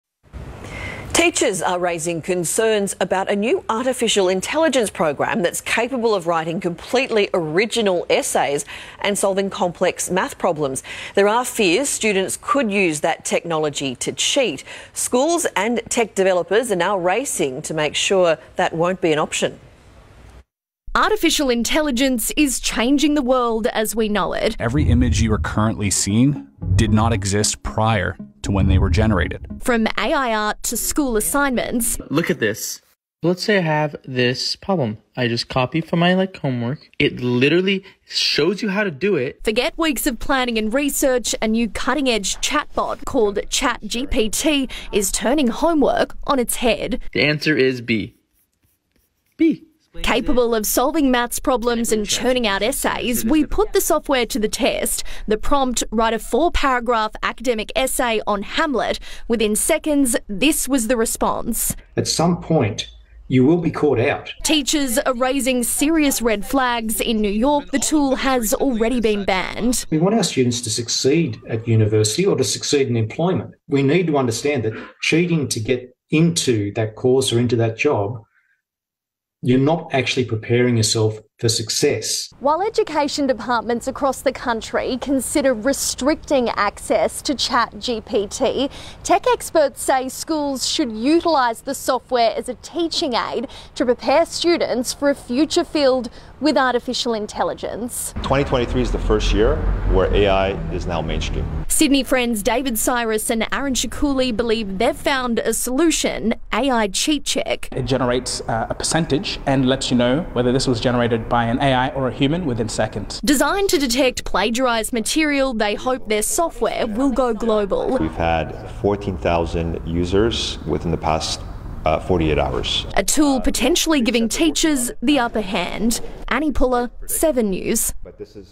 Solution to Preventing Cheating with Artificial Intelligence in Schools - Channel 7 News
Teachers are concerned about a new artificial intelligence program called ChatGPT that can write original essays and solve complex math problems. There are fears that students could use this technology to cheat on school assignments. Tech developers are working to prevent this from happening. Some schools have already banned the use of ChatGPT. Experts suggest that instead of restricting access to the program, schools should use it as a teaching aid to prepare students for a future field with artificial intelligence. A new software called AI Cheat Check, which can detect plagiarized material and determine if it was generated by an AI or a human, has been developed in response to this concern. It has already gained 14,000 users within the past 48 hours.